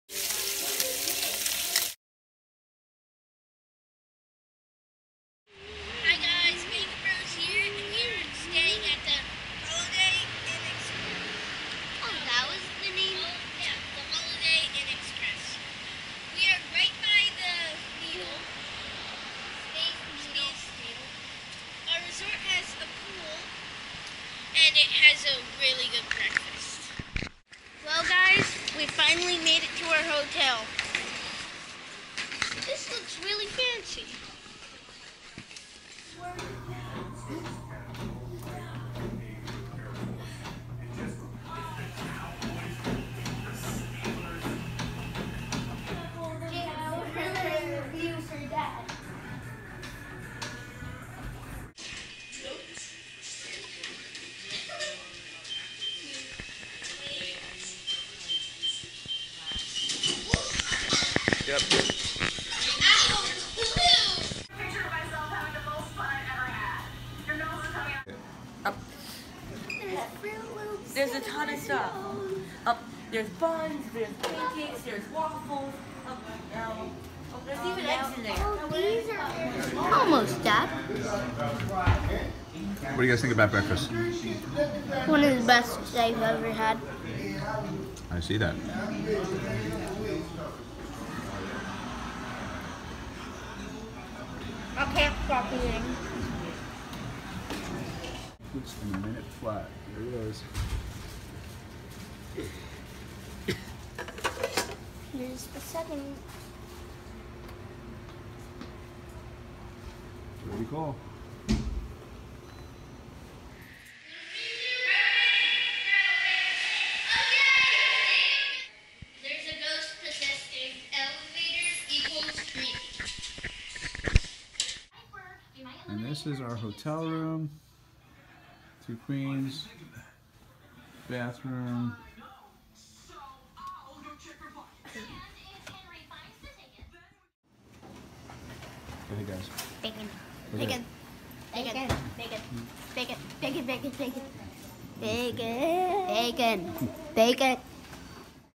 BaconBros How to Enjoy a Hotel
We went to VloggerFair this year.  This is Video 3 which is some of the highlights of staying at the Holiday Inn Express near the Space Needle.  They had the best breakfast ever and a really fun exercise room.  I hope you enjoy!

From

BaconBros Videos